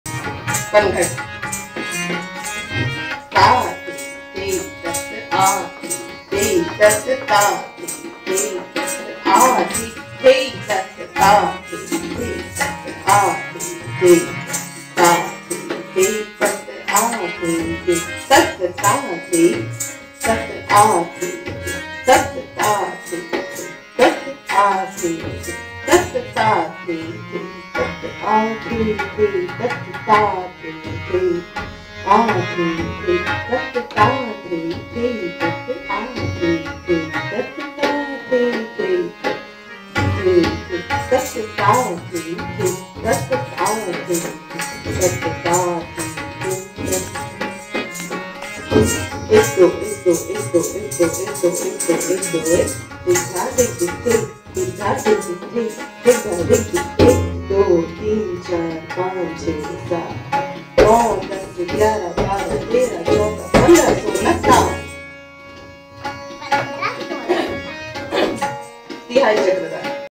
Baby Yukta Kathak Perform
Nagma (LEHRA) Playing SURAJ
Tabla PEERAJ GURUJI